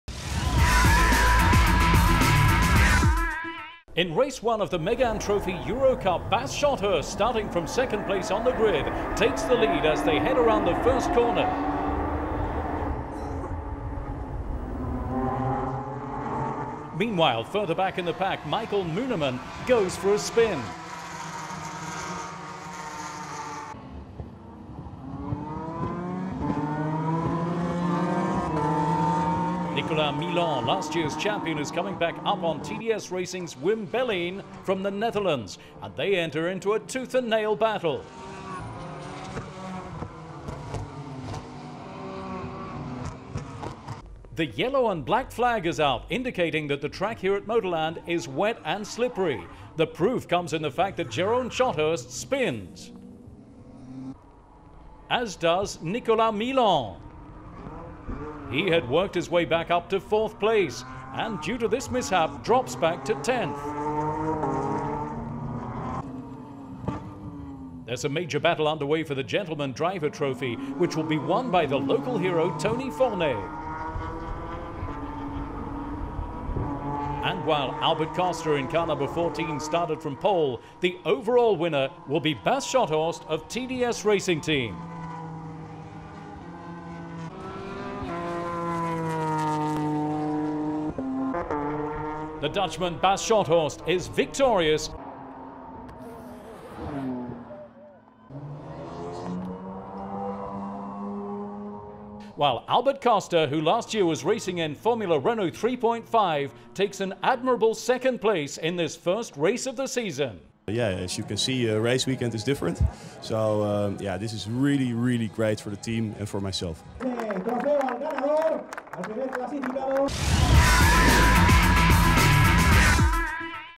WSR メガーヌトロフィー モーターランド レース1
ワールド・シリーズ・バイ・ルノー・メガーヌトロフィーのモーターランド戦レース1。